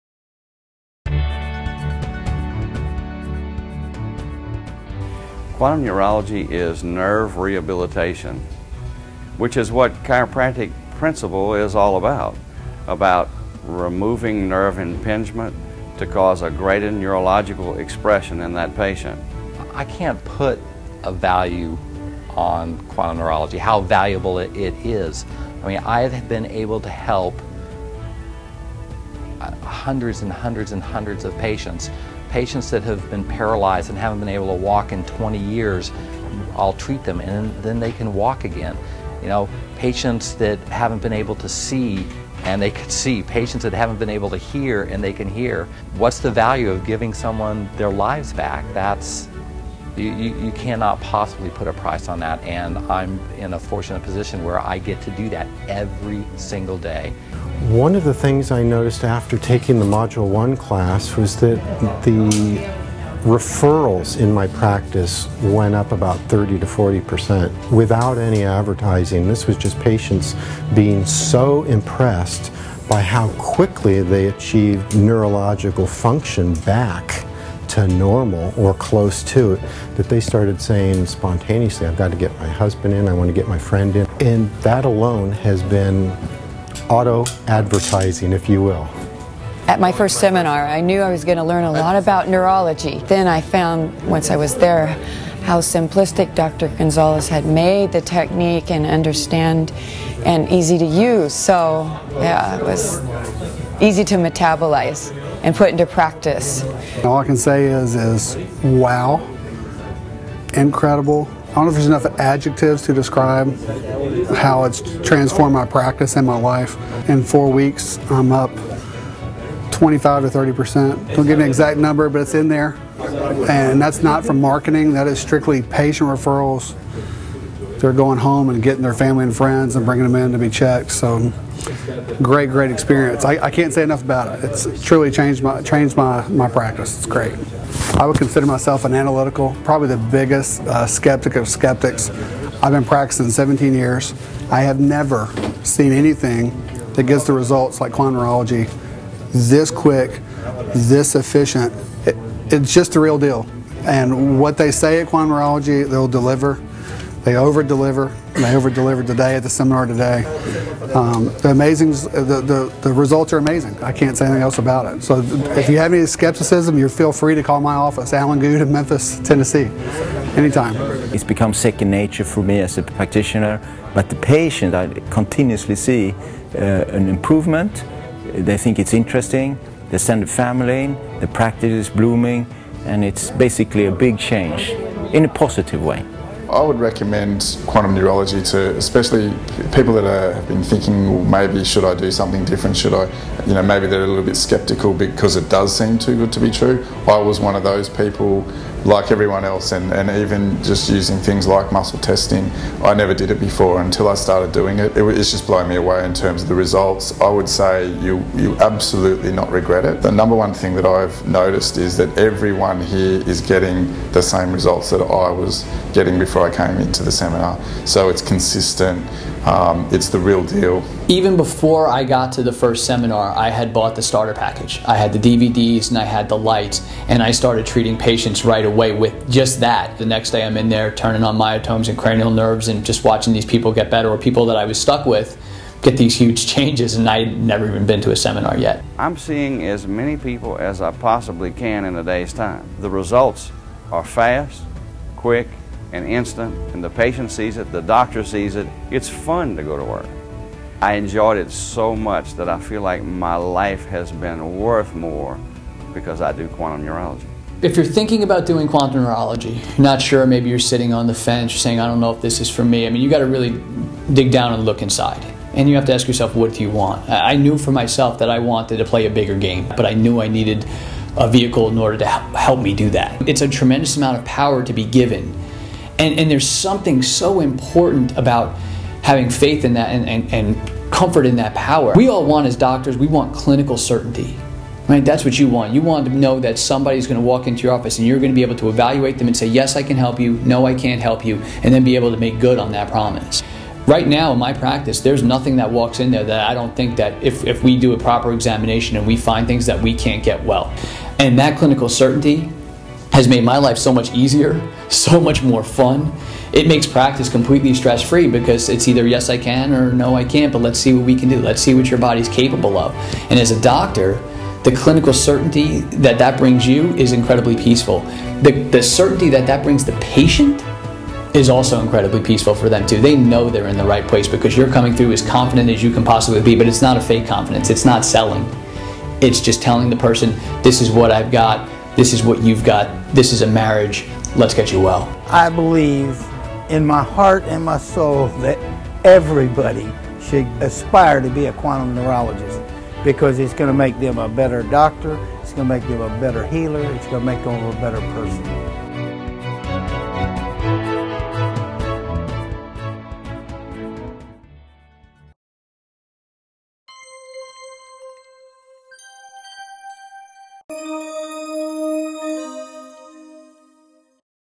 Quantum Neurology® Doctor to Doctor
In this short video several doctors share their experiences with Quantum Neurology® Nervous System rehabilitation.